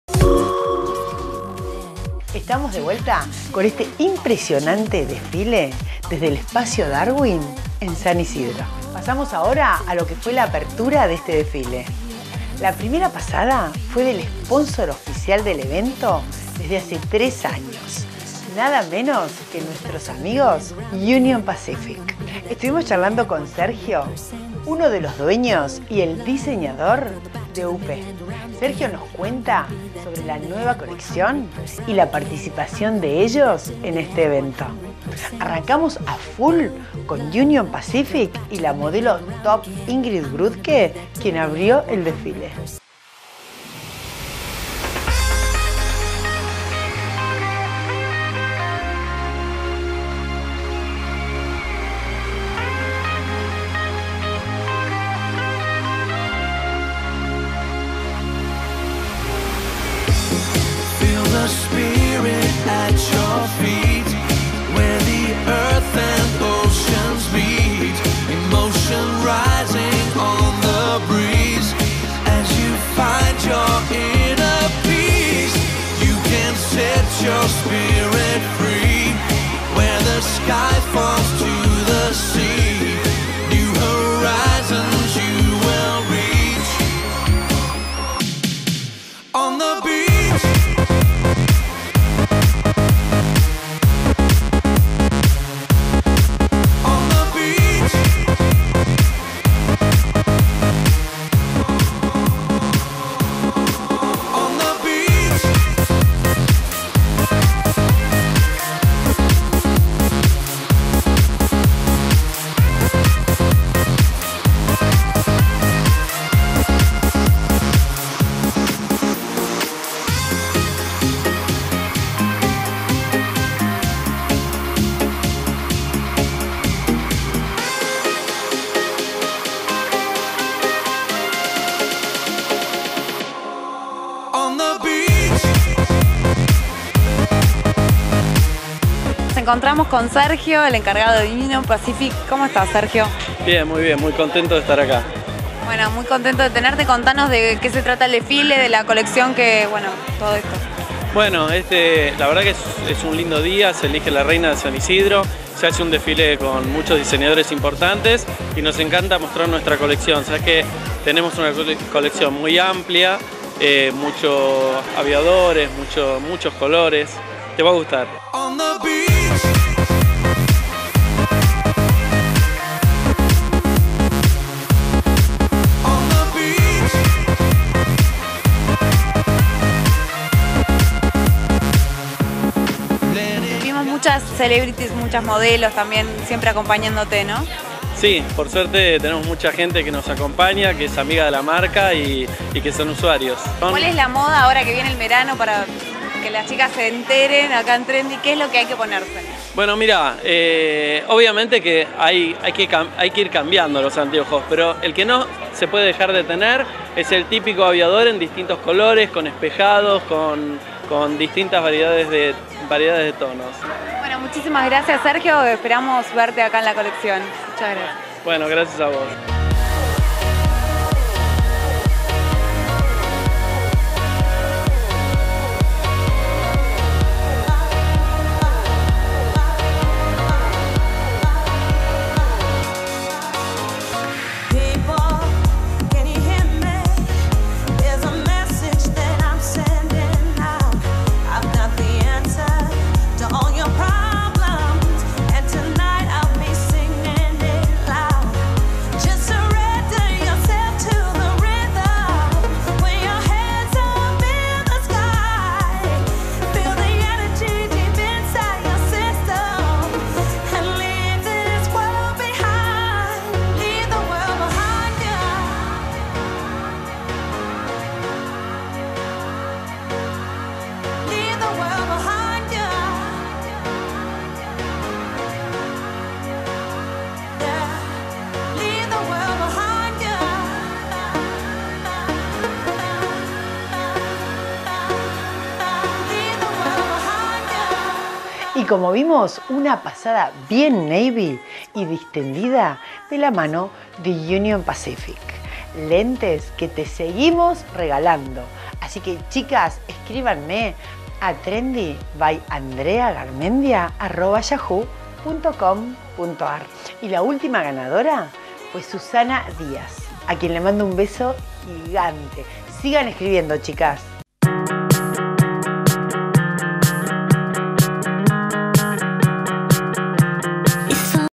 TRENDY by andrea garmendia 16 MISS SAN ISIDRO MODA 2011 2
Trendy. Miss San Isidro Moda 2011. Eleccion de la Reina. Jurado Cocho Lopez Anama Ferreira Gonzalez Oro Adriana Brosky Ricardo Piñeyro Amalia Fortabat. Backstage modelos top. Ingrid Grudke Luli Fernandez Sofia Samolo Carolina Baldini Carolina Oltra. Entrevista diseñadores. Benito Fernandez Claudio Cosano Matices Union Pacific Conduccion Dolores Barreiro Organizacion Susana Clur. Intendente Gustavo Posse. Espacio Darwin Hipodromo.